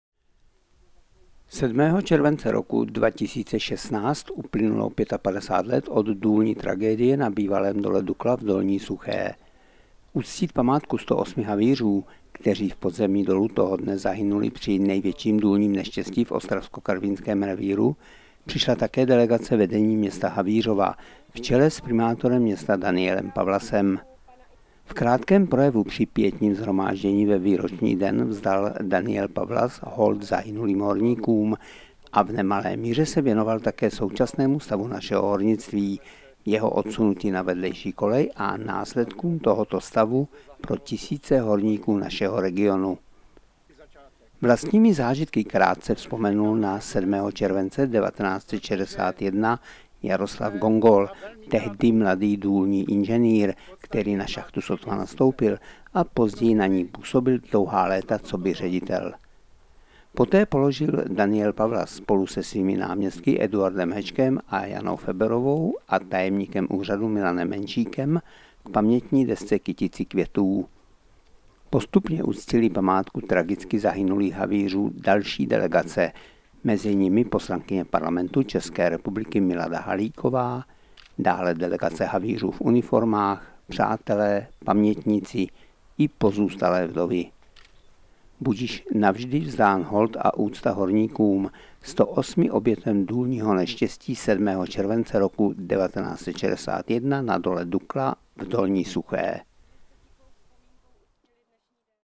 55 let od tragédie na Dole Dukla - 7. července
7. července 1961 došlo na Dole Dukla v Dolní Suché k tragickému důlnímu neštěstí, při němž zahynulo 108 horníků. Účastníci pietního shromáždění ve výroční den tragédie uctili jejich památku po 55 letech u pamětní desky u Dolu Dukla.